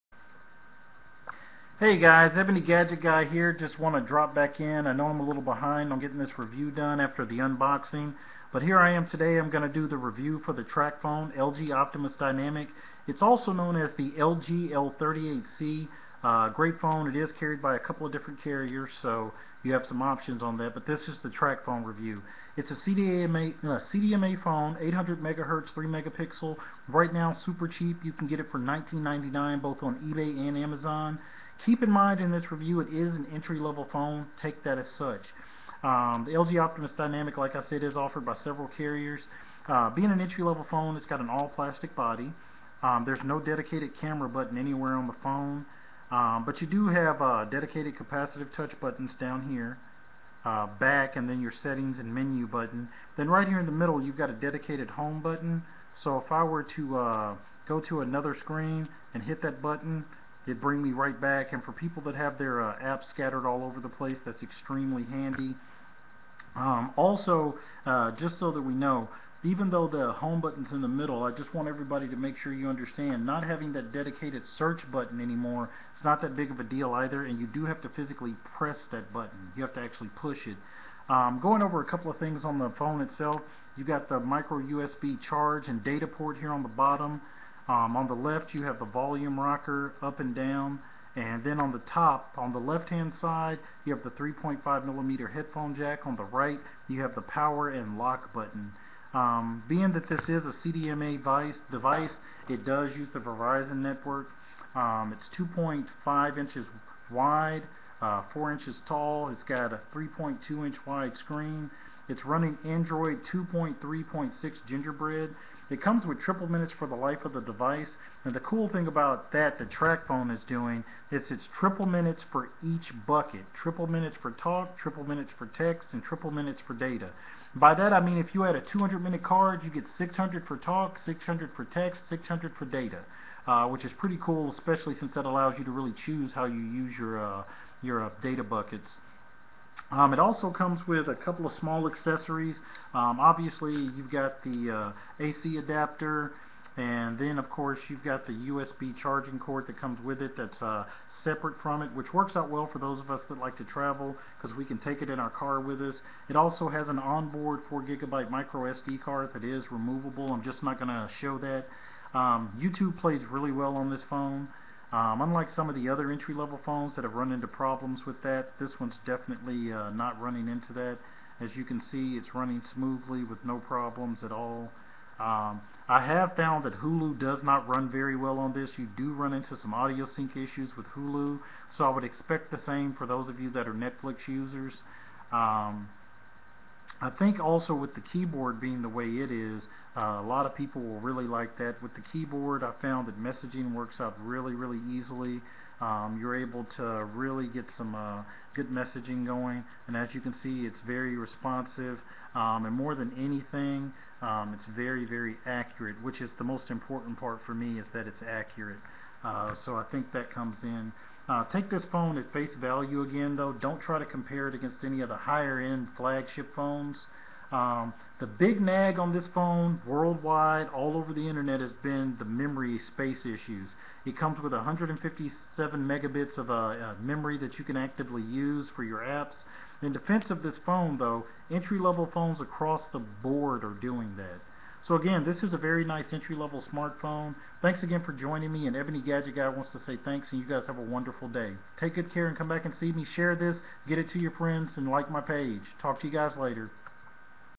Tracfone LG Optimus Dynamic Review
This is just a very brief review of  the Tracfone LG Optimus Dynamic, also known as the LG 38C